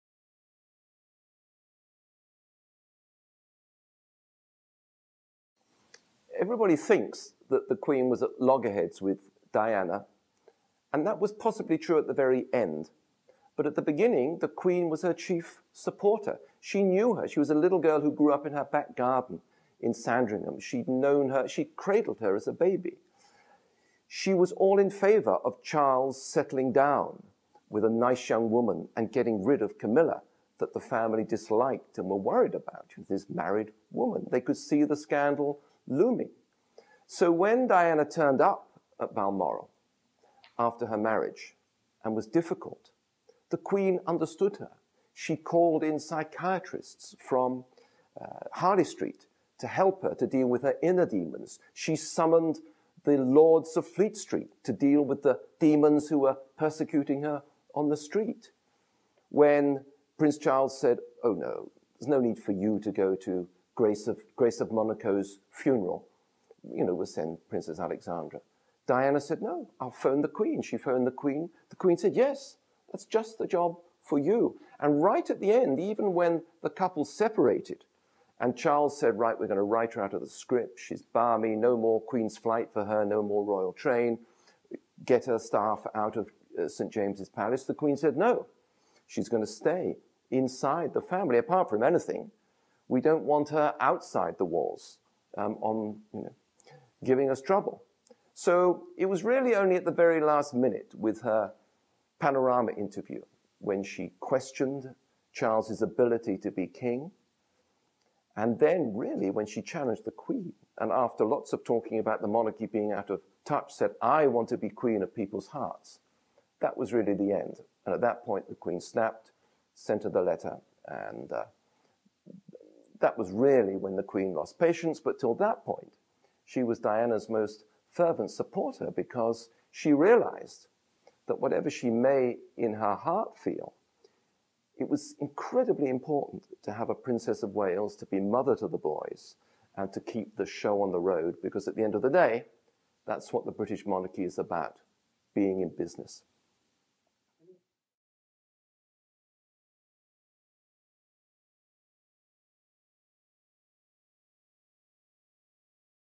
Robert Lacey The Queen and Diana US EDITION
Royal biographer Rober Lacey talking about Her Majesty Queen Elizabeth and her relationship with Diana.